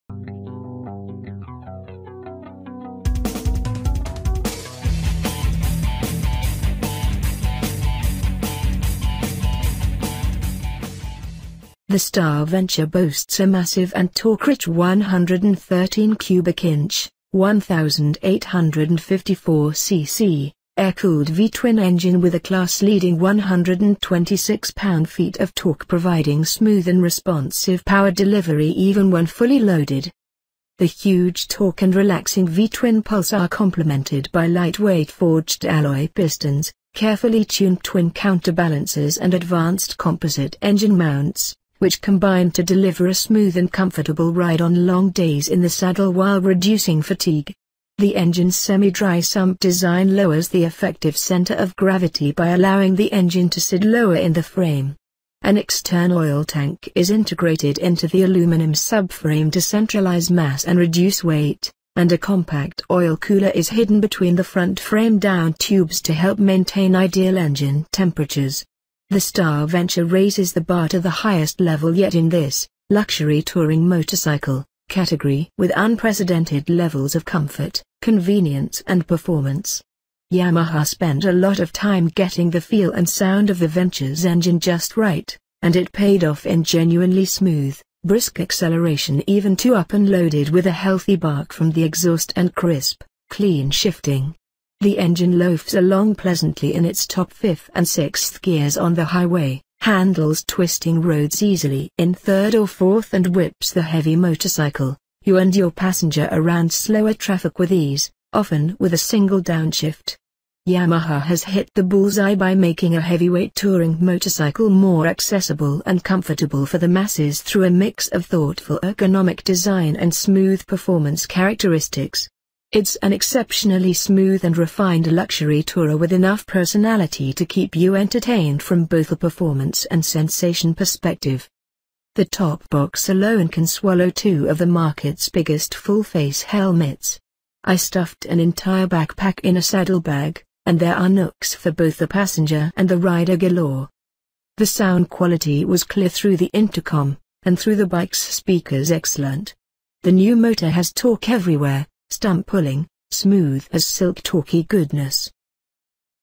2018 Yamaha Star Venture First Ride
The Star Venture boasts a massive and torque-rich 113 cubic inch (1854cc) air-cooled V-twin engine with a class-leading 126 pound-feet of torque providing smooth and responsive power delivery even when fully loaded. The huge torque and relaxing V-twin pulse are complemented by lightweight forged alloy pistons, carefully-tuned twin counterbalancers and advanced composite engine mounts, which combine to deliver a smooth and comfortable ride on long days in the saddle while reducing fatigue. The engine’s semi-dry-sump design lowers the effective center of gravity by allowing the engine to sit lower in the frame. An external oil tank is integrated into the aluminum subframe to centralize mass and reduce weight, and a compact oil cooler is hidden between the front frame downtubes to help maintain ideal engine temperatures.

“The Star Venture raises the bar to the highest level yet in this [luxury touring motorcycle] category with unprecedented levels of comfort, convenience and performance.”
 “Yamaha spent a lot of time getting the feel and sound of the Venture’s engine just right, and it paid off in genuinely smooth, brisk acceleration even two-up and loaded with a healthy bark from the exhaust and crisp, clean shifting.”
“The engine loafs along pleasantly in its top fifth and sixth gears on the highway, handles twisting roads easily in third or fourth and whips the heavy motorcycle, you and your passenger around slower traffic with ease, often with a single downshift.”

“Yamaha has hit the bullseye by making a heavyweight touring motorcycle more accessible and comfortable for the masses through a mix of thoughtful ergonomic design and smooth performance characteristics.”
“It’s an exceptionally smooth and refined luxury tourer with enough personality to keep you entertained from both a performance and sensation perspective.”
“The top box alone can swallow two of the market’s biggest full face helmets. I stuffed an entire backpack in a saddlebag, and there are nooks for both the passenger and the rider galore.”
“The sound quality was clear through the intercom, and through the bike’s speakers excellent.”
“The new motor has torque everywhere, stump-pulling, smooth as silk torquey-goodness.”